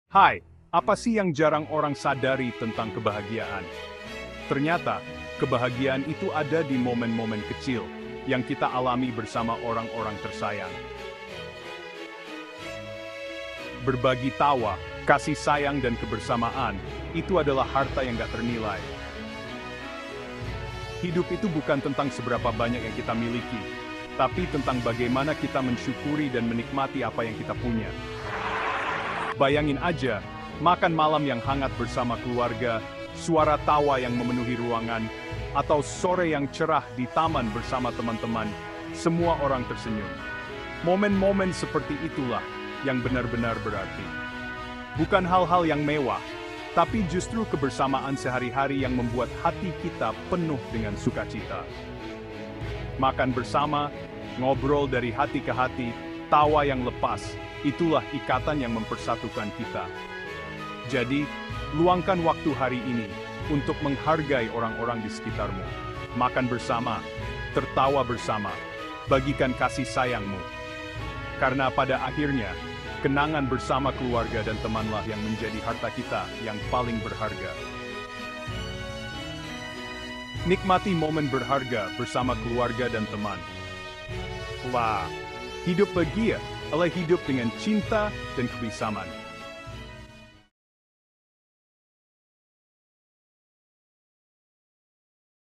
~Apa sih Kebahagiaan?~
"Berbagi tawa, cinta, dan kebersamaan dengan orang-orang terdekat, itu adalah harta yang paling berharga. Hidup bukan tentang berapa banyak yang kita miliki, tetapi tentang bagaimana kita menikmati apa yang kita punya."

arti kebahagiaan
sederhana bahagia
makna bahagia